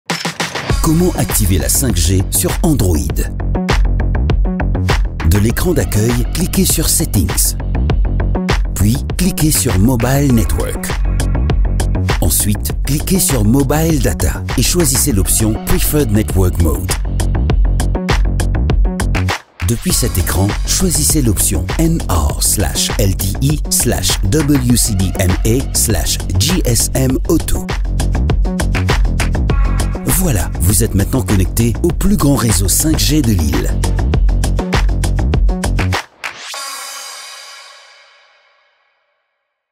How to activate 5G on your Huawei 📲
Kouma pou konekte lor pli gran rezo 5G dan Moris lor to Huawei ? 📲

Fasil net! Swiv bann etap 👇

Assurez vous d’avoir un smartphone Huawei compatible à la 5G et une carte sim Emtel 😉

Disponib lor tou pack Mobile Data Emtel ‼ Si nou dir zot li pli rapid, ki rapid!